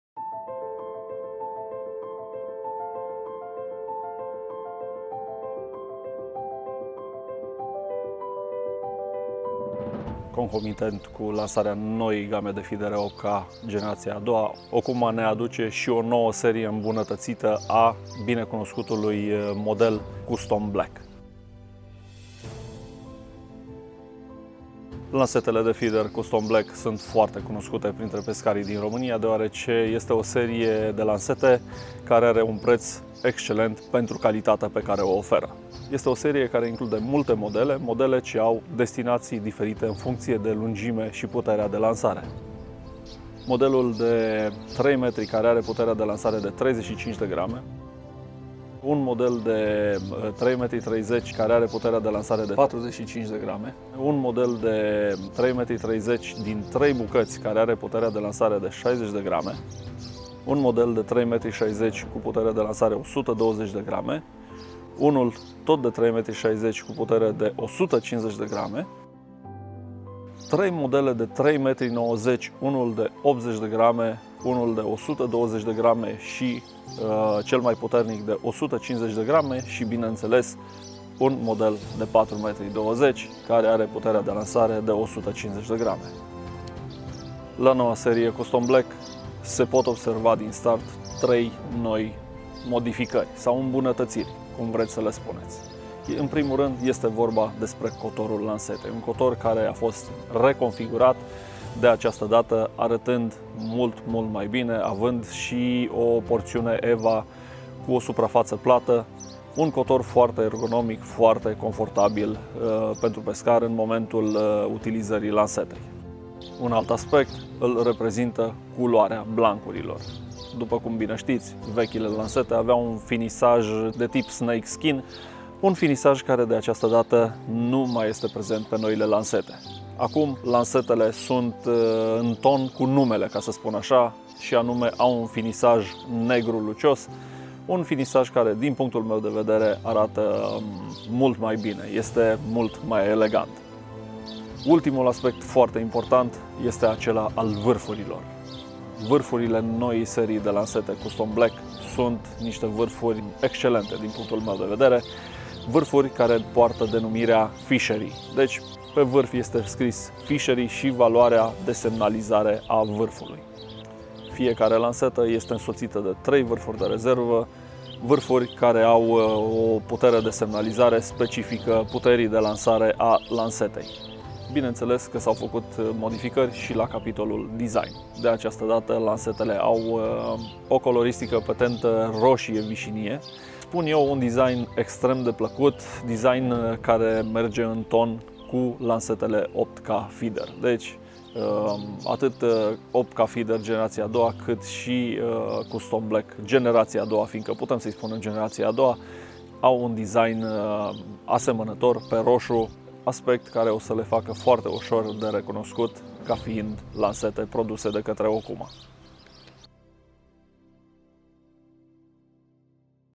Okuma Custom Black Feeder 2024- Prezentare generala
Gama de lansete feeder Okuma Custom Black este o gama foarte cunoscuta in randul pescarilor de feeder din Romania. In acest an 2024 producatorul Okuma reformeaza seria Custom Black, reusind astfel sa imbunatateasca performantele acestor lansete. In seria clipurilor de prezentare a noii serii Okuma Custom Black fac parte trei clipuri: prezentare generala, prezentare modele de 3.00mt/3.30mt/3,60mt si prezentare a modelelor lungi de 3.90mt si 4.20mt.